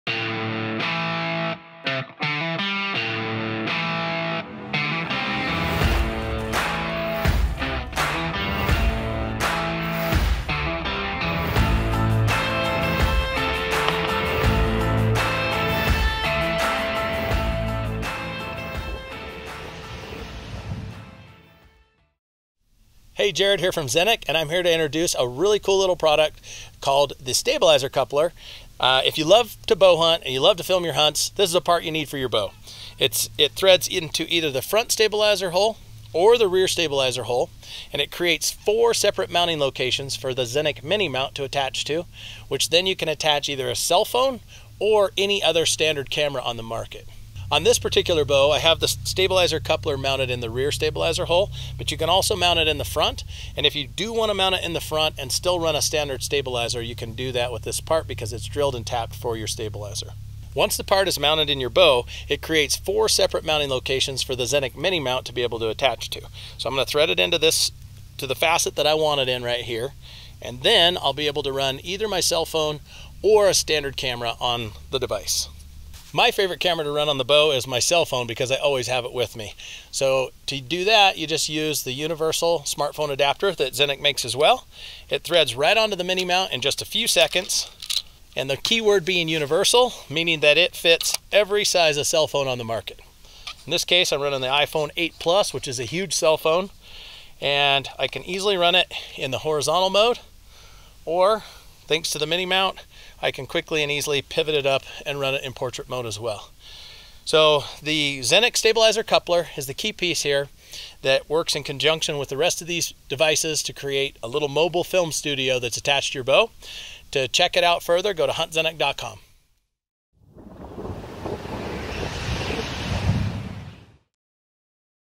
Film Your Hunt - Bow Bundle Options
The Lightest, Most Compact, and Most Versatile Solution on the Market for Bow Mounted Filming Solutions